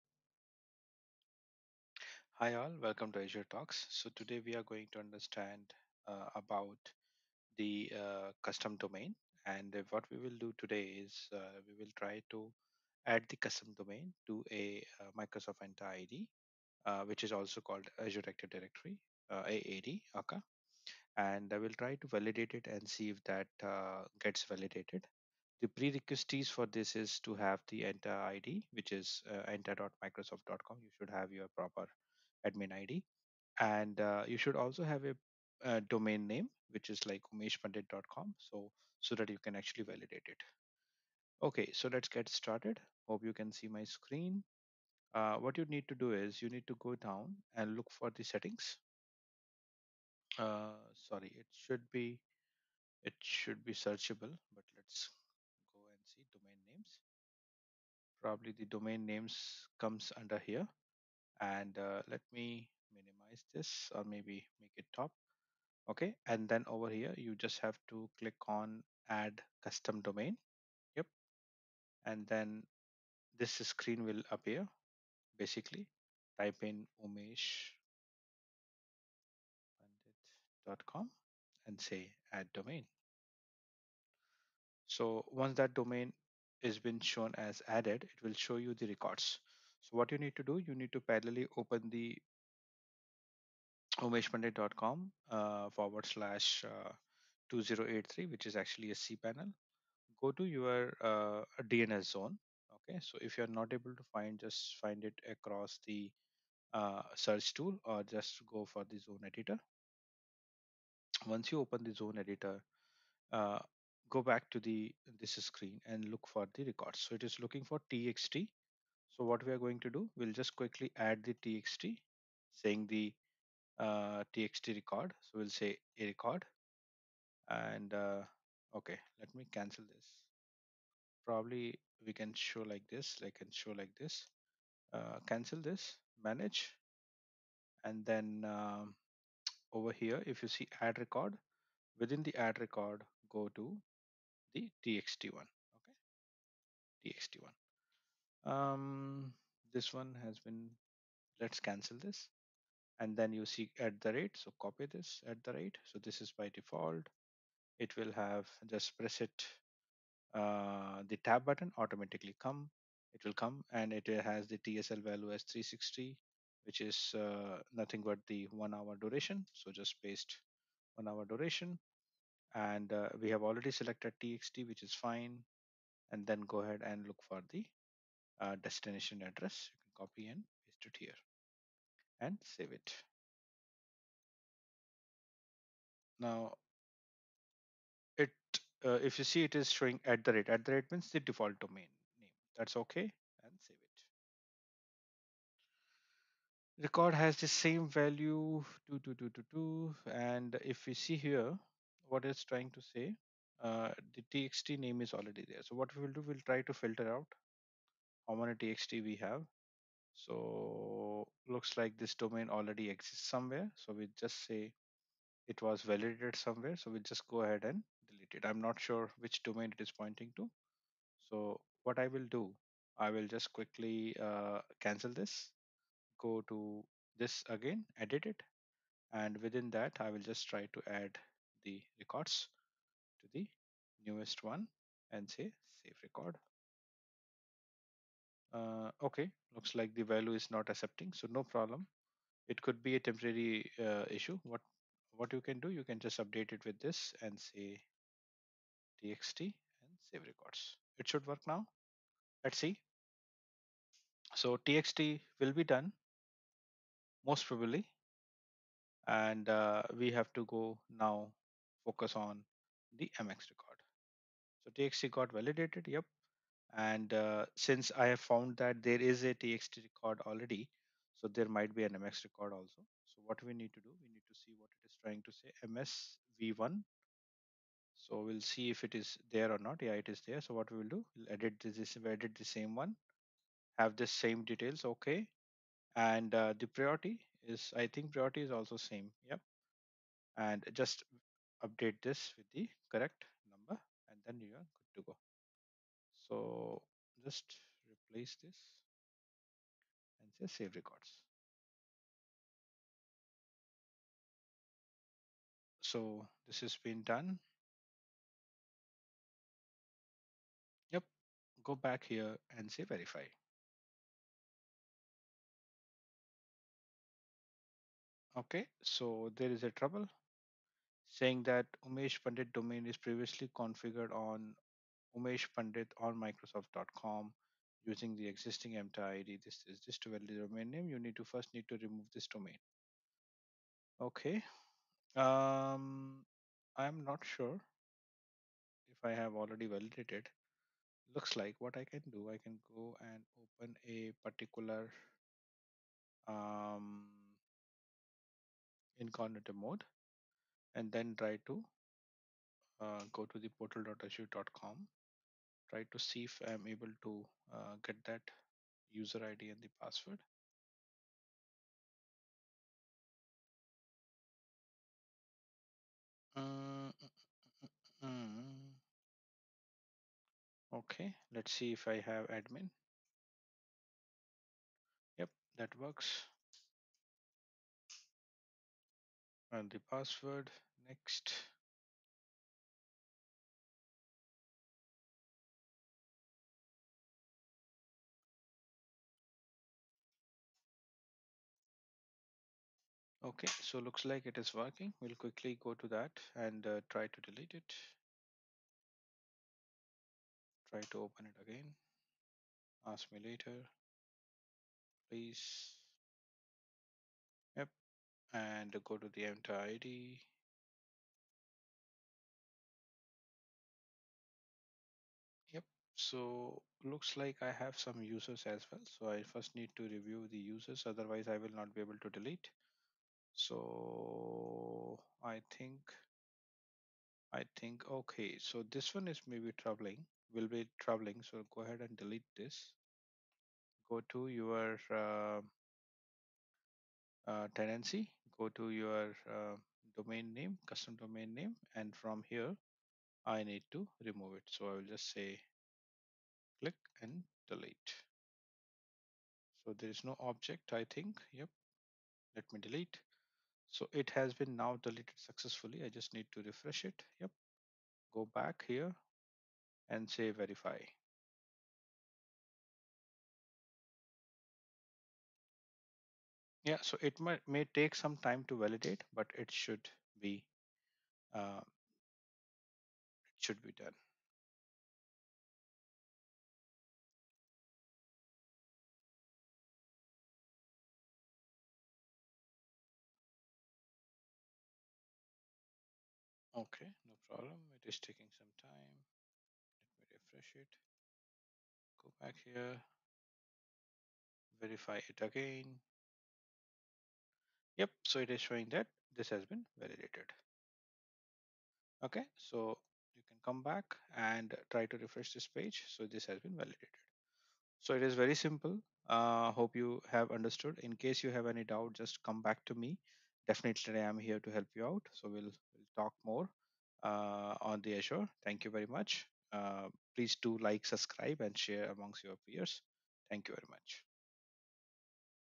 Managing custom domain names in your Microsoft Entra ID
Here are the steps to add a custom domain name to your Microsoft Entra ID:
1. Sign in to the Microsoft Entra admin center as at least a Domain Name Administrator.
2. Browse to Identity select Settings, click Domain names then Add custom domain.
3. In Custom domain name, enter your organization’s domain.
4. Select Add domain.
5. The unverified domain is added. The page appears showing the DNS information needed to validate your domain ownership.